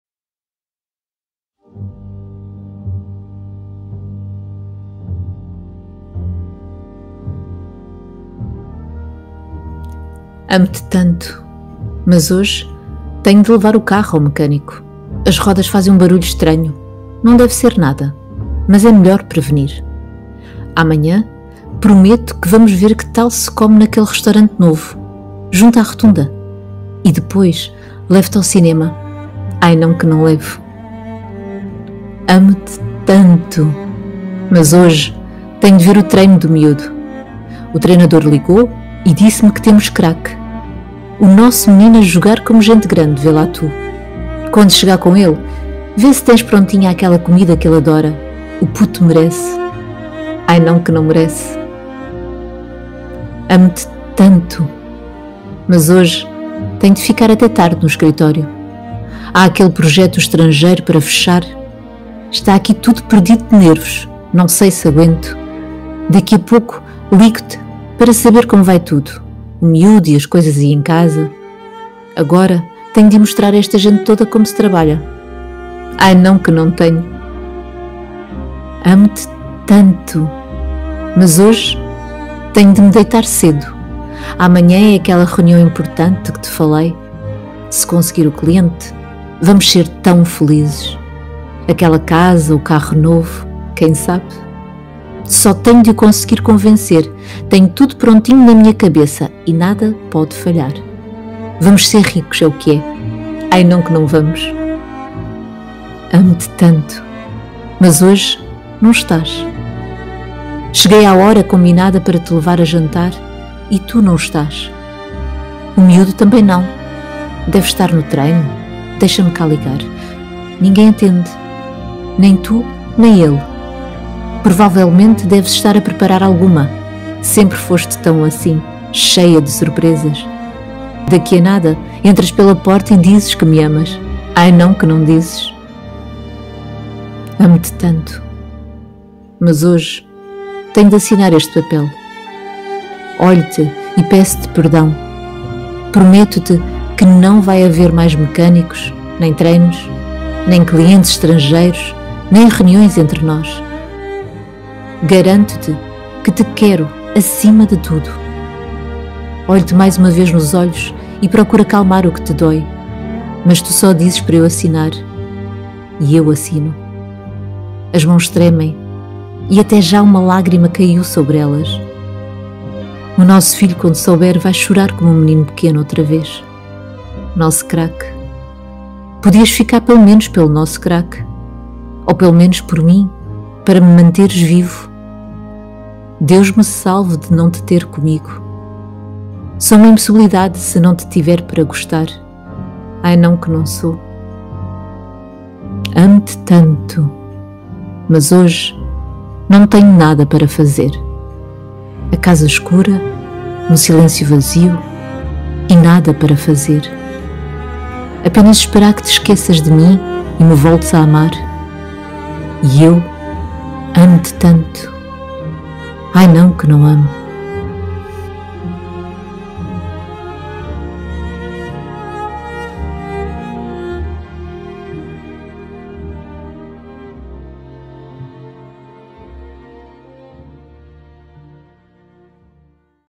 Amo-te Tanto | Poema de Pedro Chagas Freitas com narração de Mundo Dos Poemas
Poesia e poema de autor português. Pedro Chagas Freitas ama e é amado. Escreve quando pode e às vezes quando não pode. Escreveu centenas de livros e publicou 31. Vendeu mais de um milhão de cópias em todo o mundo. As suas obras estão traduzidas para mais de uma dezena de línguas e espalhadas por mais de meia centena de países. Está entre os autores mais vendidos no México, em Itália, no Brasil e em Portugal. Inventou jogos de escrita. Cria e coordena workshops de escrita criativa. Mas acima de tudo ama e é amado.

link do vídeo Visite a nossa loja de recordações e tenha um pouco de nós perto de si.